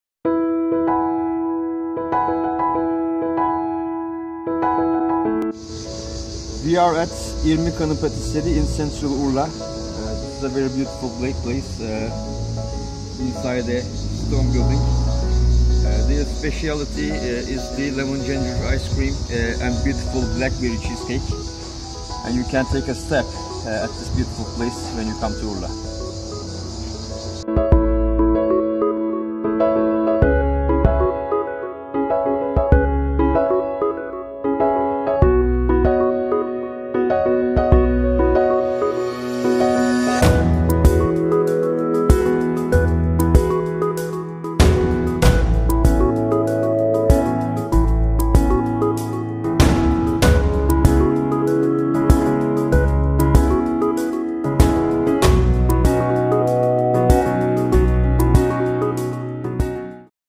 Irmik Hanim Patisserie - Urla, Izmir
Irmik Hanim is a small patisserie inside a stone house in central Urla, with delicious French deserts and goat milk ice cream.